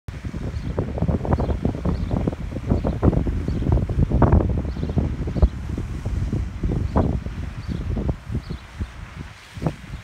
The Ducks at Swan Club On The Harbor
Ducks love to swim and play in our waterfront gardens.